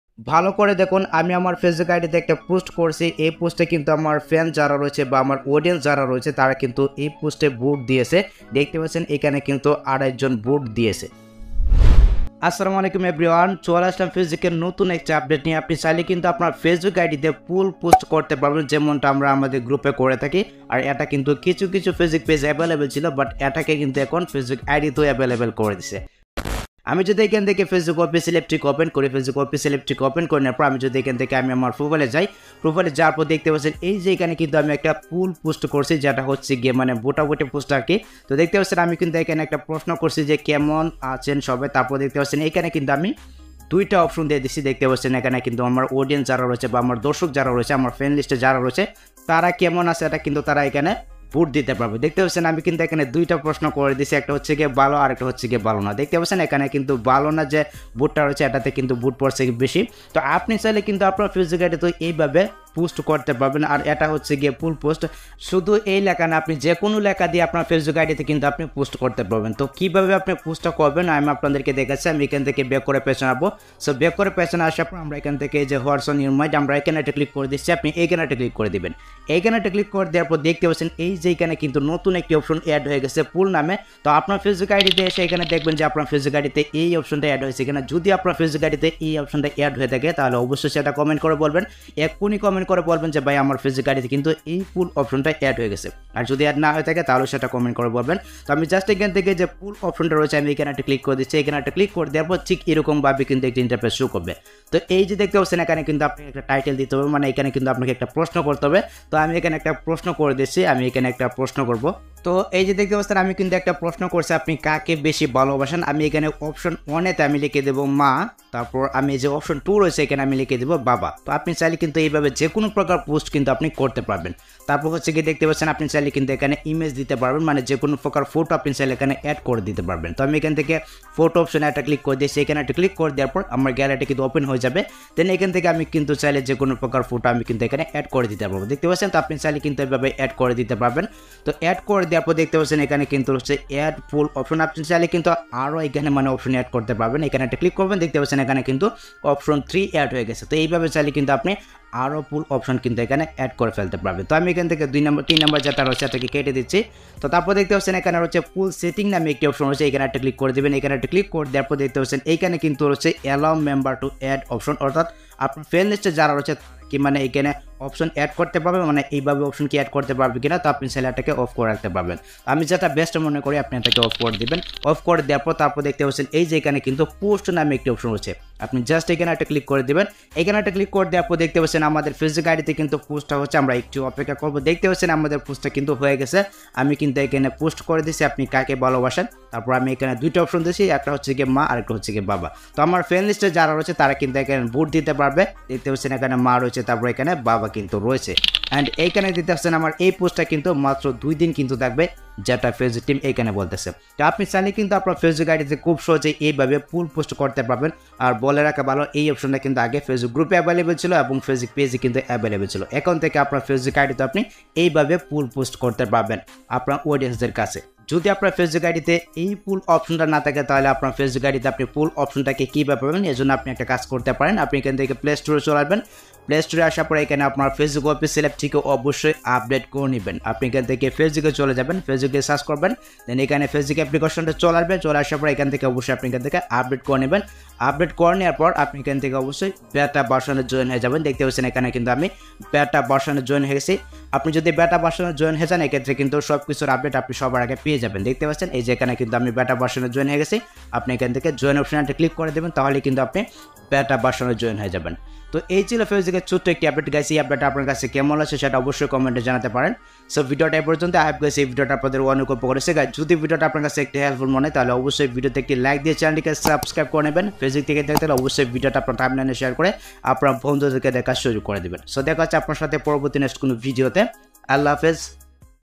Facebook new update | How to create poll post on facebook account || Facebook profile poll post 2023
💌 আপনার যেকোনো সমস্যা সমাধানের জন্য আমাদের ফেসবুক গ্রুপে জয়েন হয়ে পোস্ট করতে পারেন, গ্রুপ লিংক